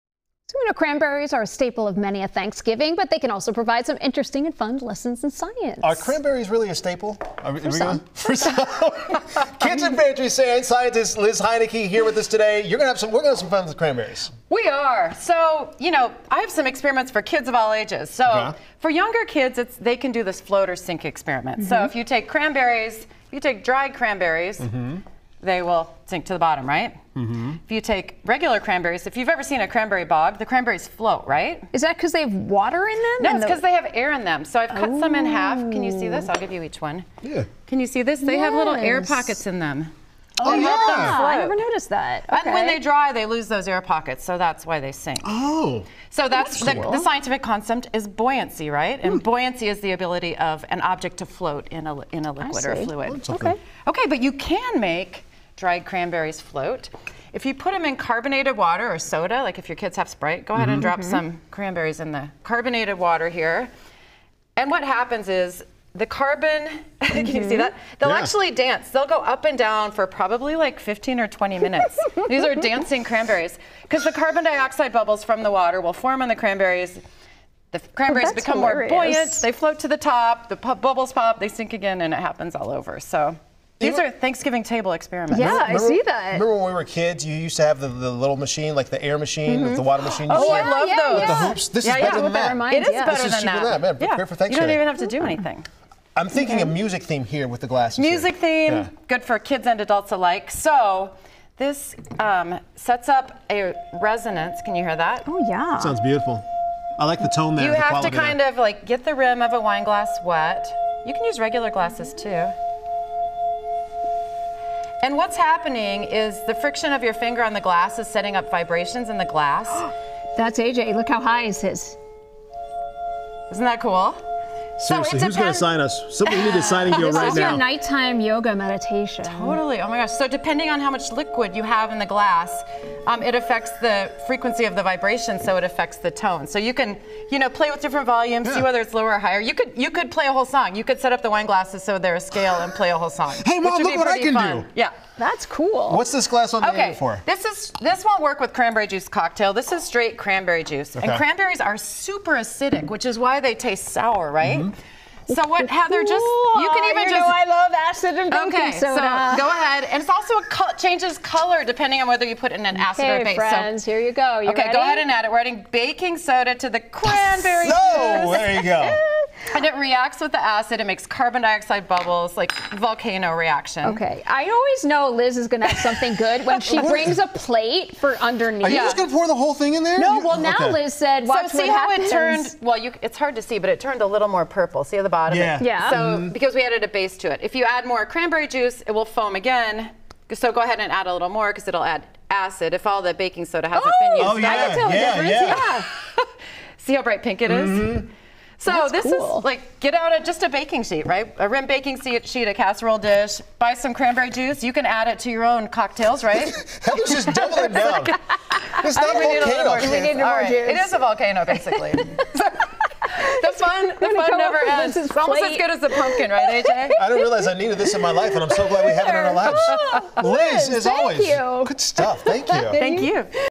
Cranberries: a Thanksgiving staple AND perfect subject for science experiments
Kitchen Pantry Scientist Liz Heinecke shows Heather Brown and A.J. Hilton the scientific side of cranberries.